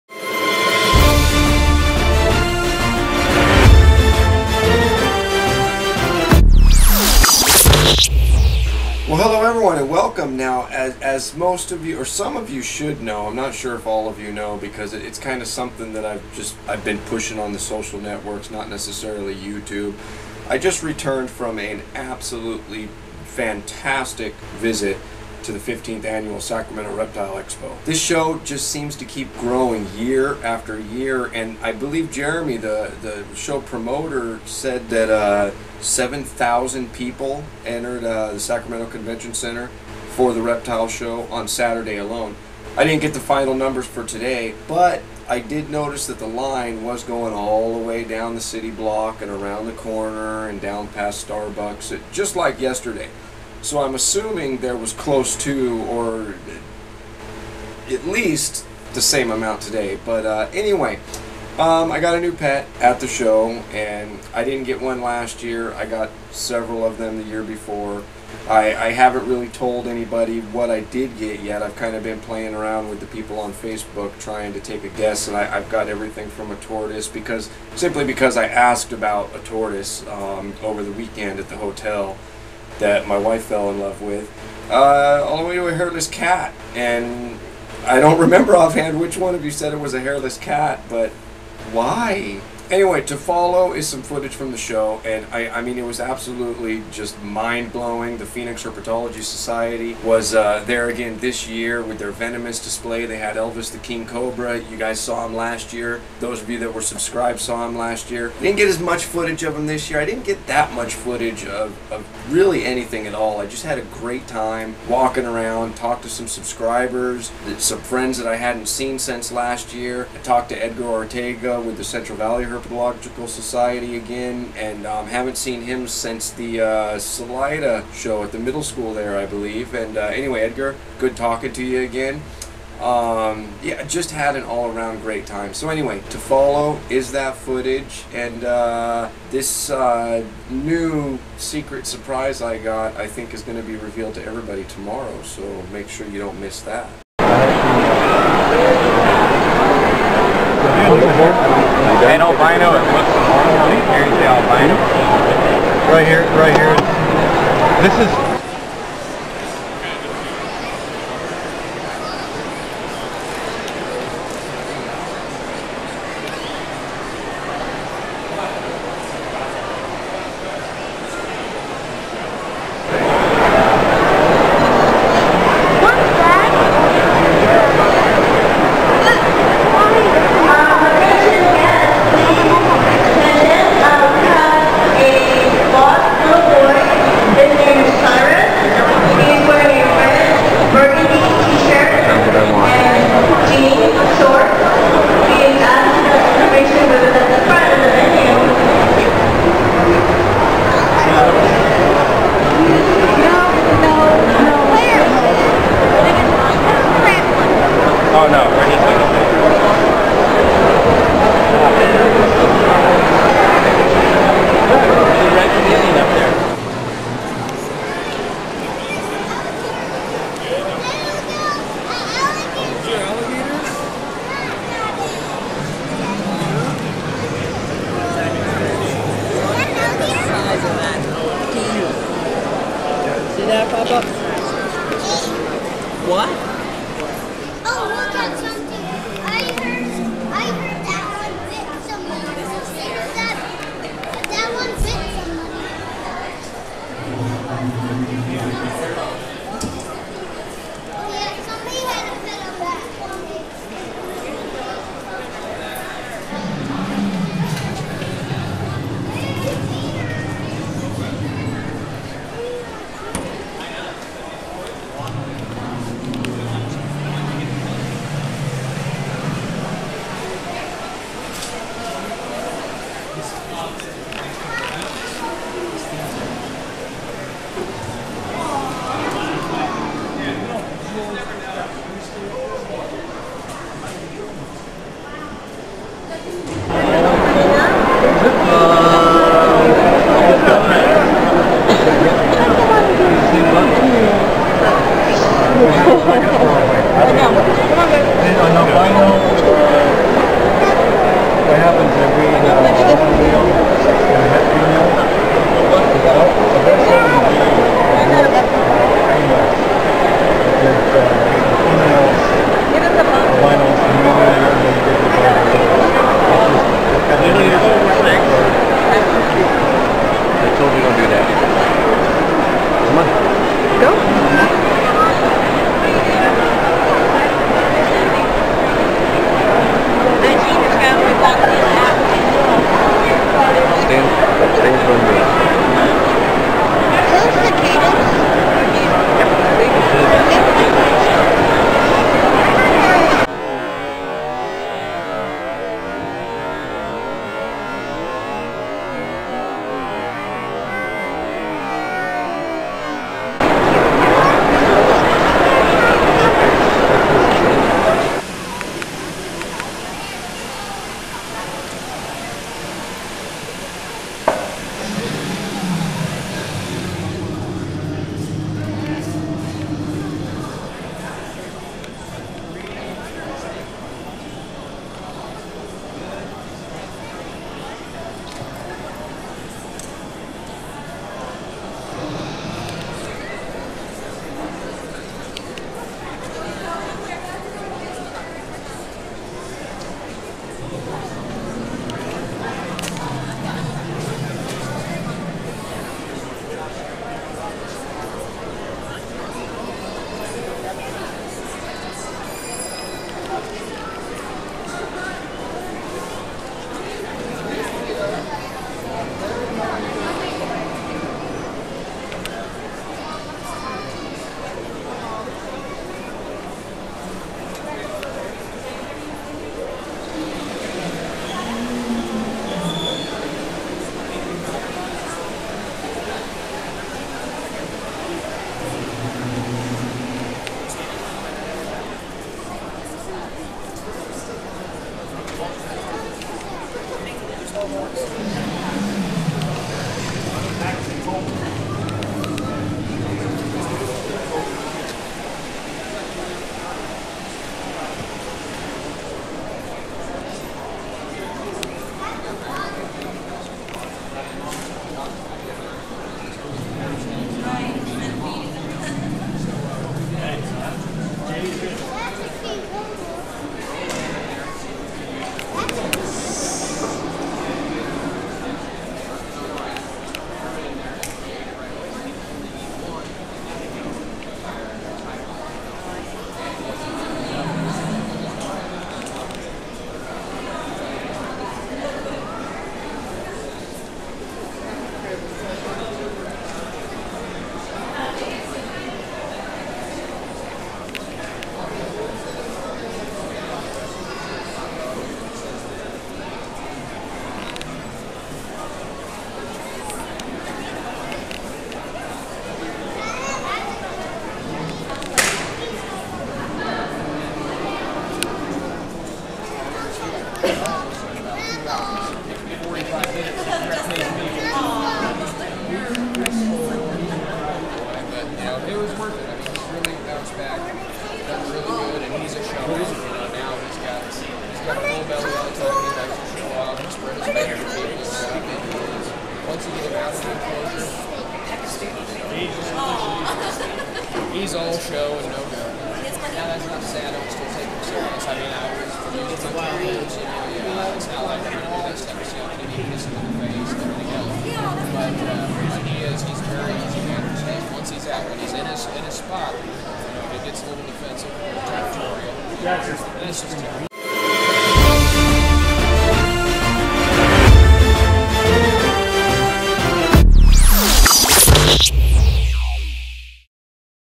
King Cobra 15th Annual Sacramento Reptile Show
We had a GREAT time at the Sacramento Reptile Show this weekend, saw thousands of animals and great people.
Information added by a viewer
something for in your video description, feel free to add.
i just recognize a few venomous there...
4:02 ophiophagus hannah
6:00  Montivipera raddei raddei
6:26 Bothriechis schlegelii
6:51 Trimeresurus albolabris
7:40 Dendroaspis augusticeps
08:05 Naja sumatrana
08:29 Naja annulifera banded
08:33 Dendroaspis polylepis